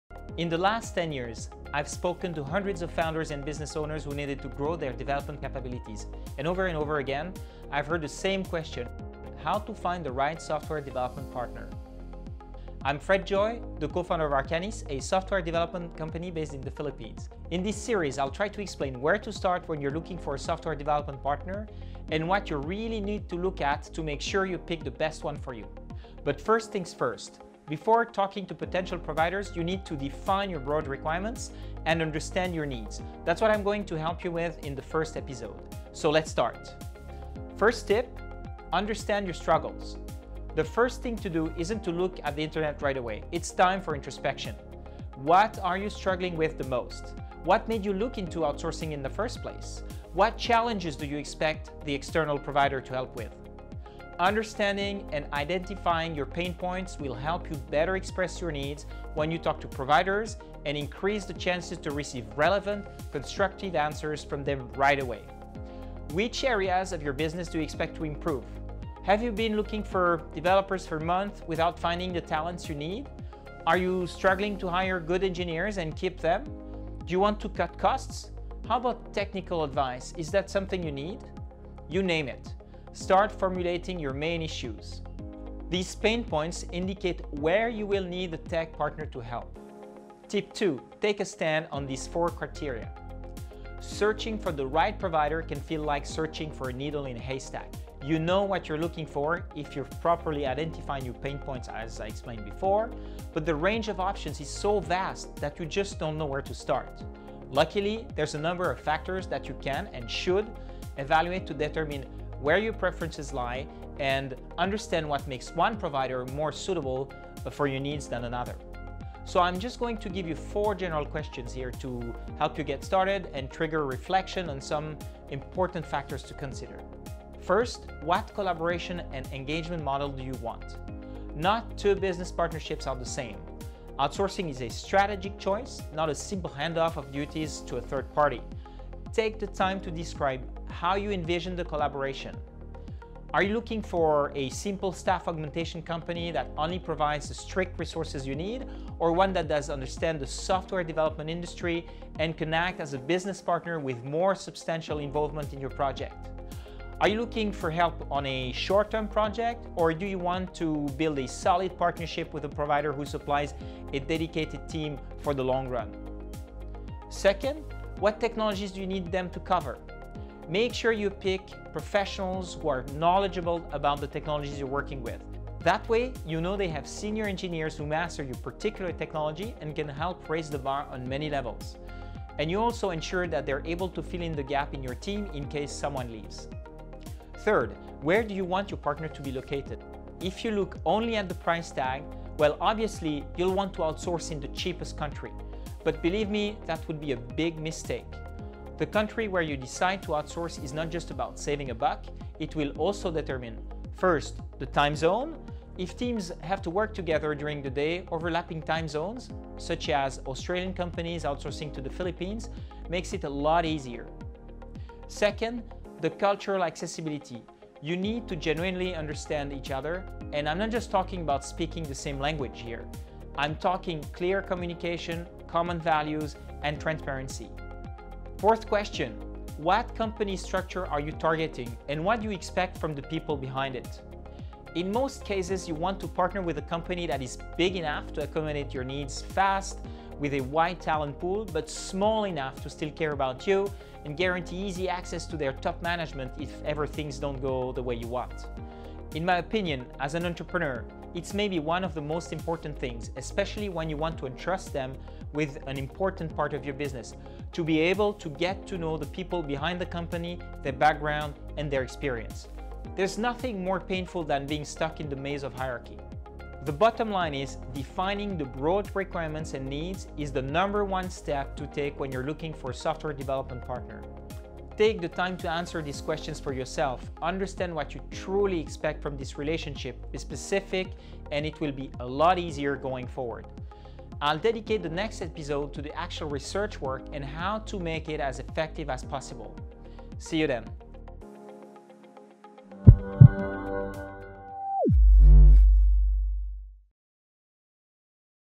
How to Define Broad Requirements for Software Development Companies
It's settled: you've decided to outsource part of your software development. But are your expectations crystal clear? How do you envision the collaboration? In this episode, I give you a rundown of criteria to reflect on to make sure you start looking for the right type of outsourcing providers.

Video Chapters
0:00 Intro to Requirements for Software Development Companies
0:37 1. Understand your Struggles
1:30 2. Take a Stand on these 4 Criteria
2:00 General Questions to Get Started and Trigger Reflection
2:07 What Collaboration and Engagement Model Do You Want?
2:47 What Technologies Do You Need Them to Cover?
3:07 Where Do You Want Your Partner to Be Located?
3:49 What Company Structure Are You Targeting? What Do You Expect from the People Behind it?

Watch the full series “2021 Guide to Evaluating Software Development Outsourcing Firms”